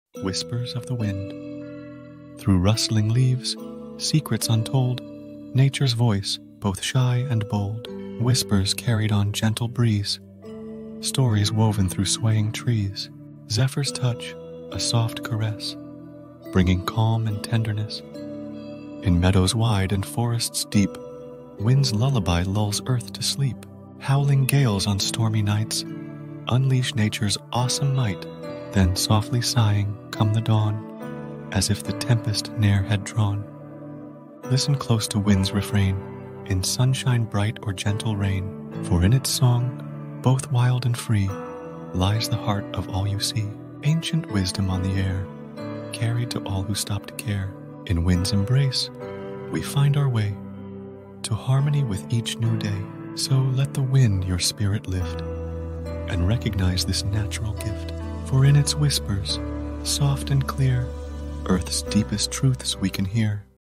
Whispers of the Wind (Poem)
- [00:00]( The wind carries untold secrets through rustling leaves, representing both a shy and bold voice of nature.
- [00:12]( Gentle breezes weave stories through swaying trees, bringing calm and tenderness in wide meadows and deep forests.
- [00:26]( Howling gales on stormy nights showcase nature's might, followed by a soft sigh at dawn.
- [00:41]( The wind's song, whether wild or gentle, holds ancient wisdom and connects us to nature's heart.
- [00:56]( Embracing the wind lifts our spirits and reveals Earth's deepest truths in its whispers.